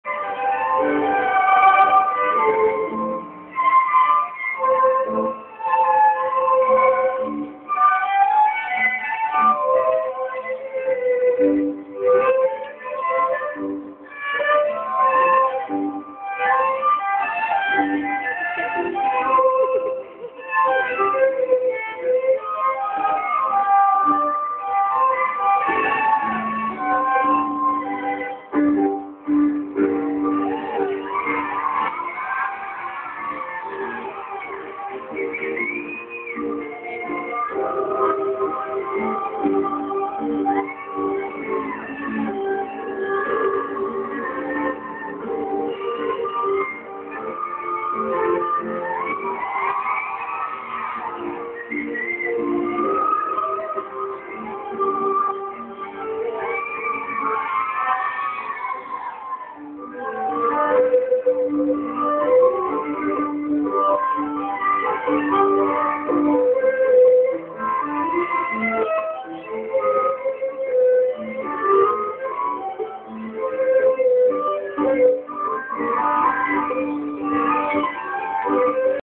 Tais e Junior dançam tango na amostra pedagogica da Escola Joana Emilia
Tais e Junior tançam tango na abertura da amostra pedagogica da Escola Joana Emilia que aconteceu nesta quinta-feira, 29 de outubro de 2009.